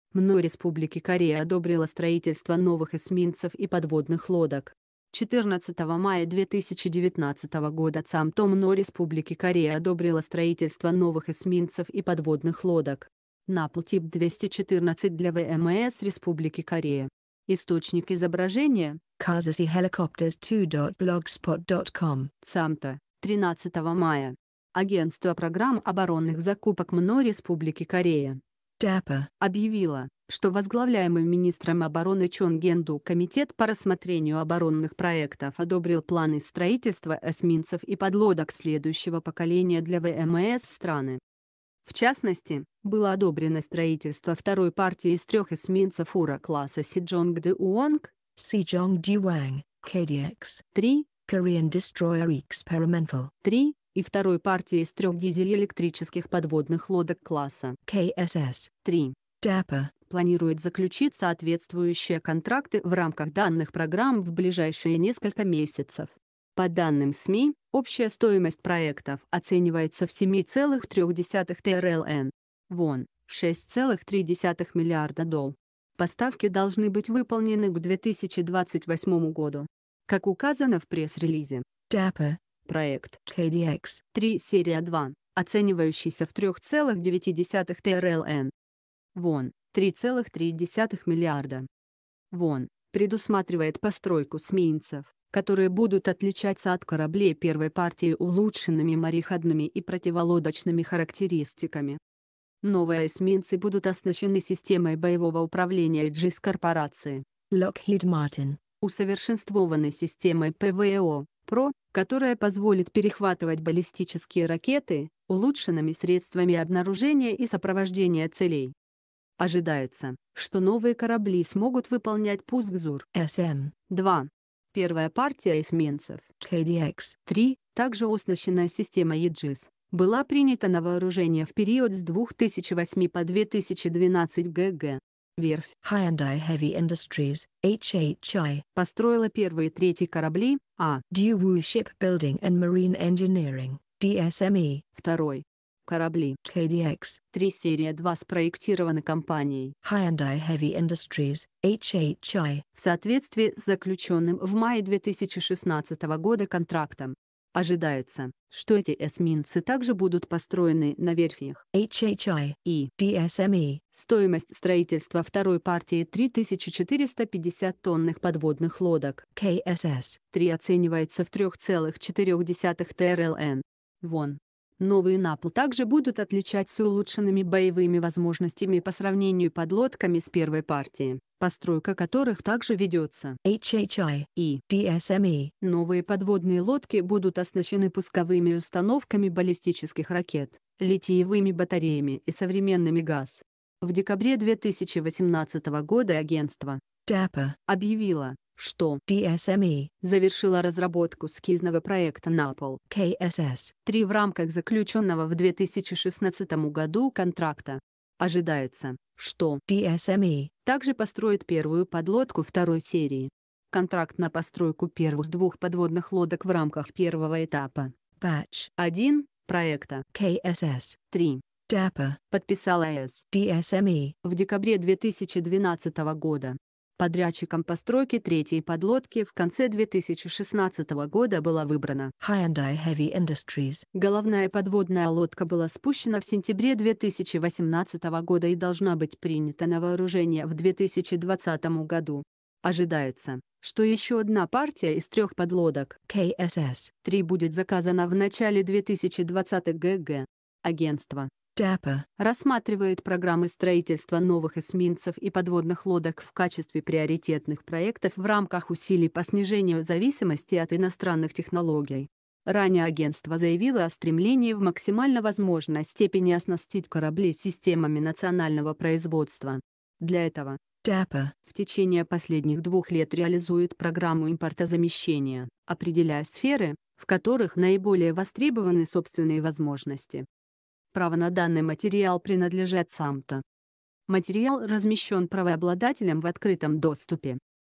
МНО Республики Корея одобрило строительство новых эсминцев и подводных лодок
МНО Республики Корея одобрило строительство новых эсминцев и подводных лодок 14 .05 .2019 ЦАМТО МНО Республики Корея одобрило строительство новых эсминцев и подводных лодок НАПЛ Тип 214 для ВМС Республики Корея . Источник изображения cazasyhelicopters2 .blogspot .com ЦАМТО, 13 мая . Агентство программ оборонных закупок МНО Республики Корея DAPA объявило, что возглавляемый министром обороны Чон Ген Ду комитет по рассмотрению оборонных проектов одобрил планы строительства эсминцев и подлодок следующ